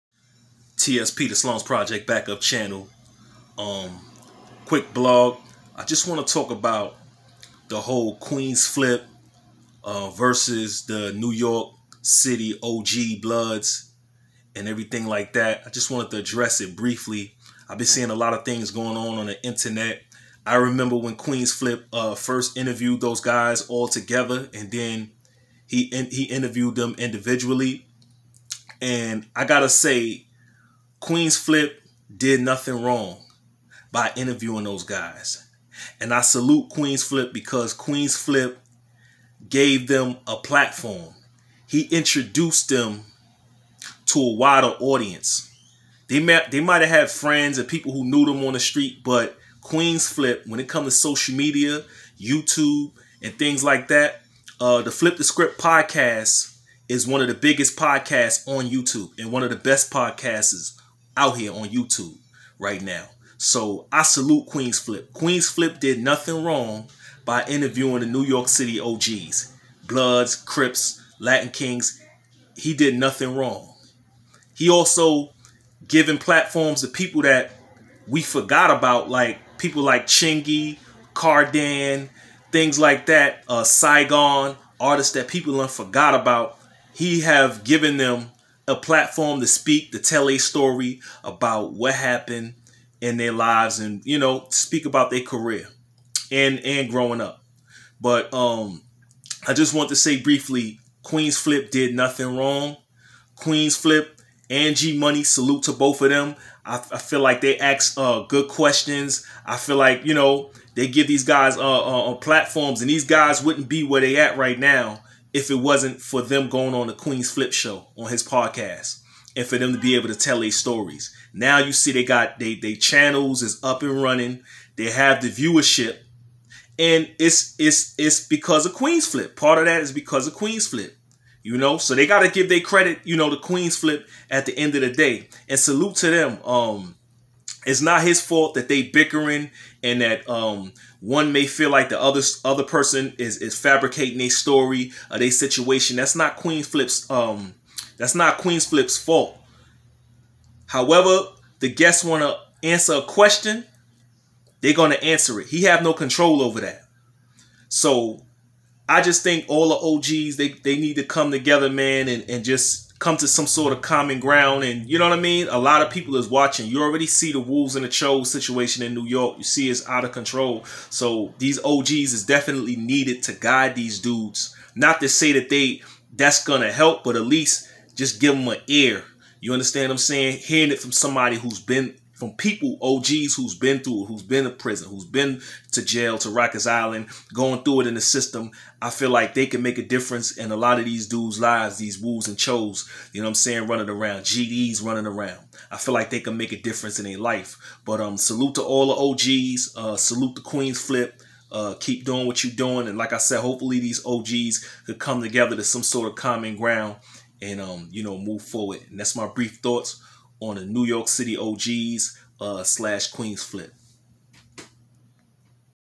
T.S.P:Queenz flip/NYC OG Bloods conflict
Street news hood politics podcaster Queenz flip from the flip the script podcast have been recently criticized for interviewing the NYC OG Bloods there is conflict amongst the OG's. My brief thoughts.